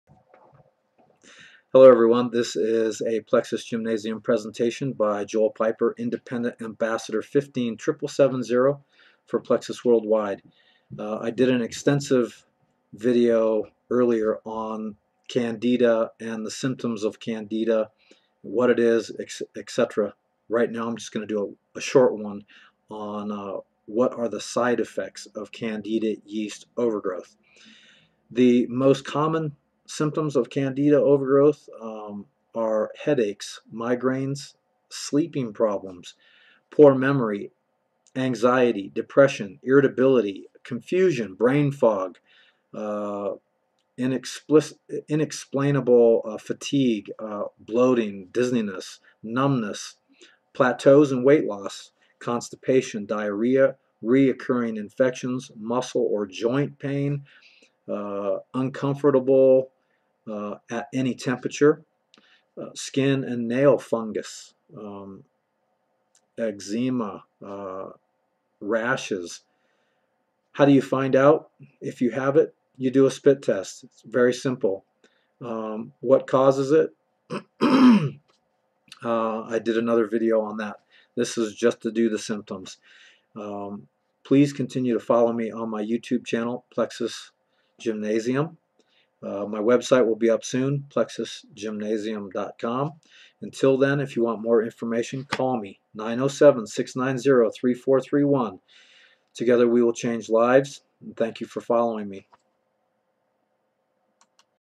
# 157770 Plexus and Candida symptoms
What Are The Side Effects Of
Candida Yeast Overgrowth?

The most common symptoms of candida
overgrowth are listed below.
 • headache • migraine • sleeping problems •
poor memory • anxiety • depression •
irritability and confusion • "brain fog" •
inexplicable fatigue or bloating • dizziness •
numbness • plateau in weight loss •
constipation • diarrhea • recurring infections •
muscle or joint pain • uncomfortable at any
temperature • skin and nail fungal infections •
eczema • rashes •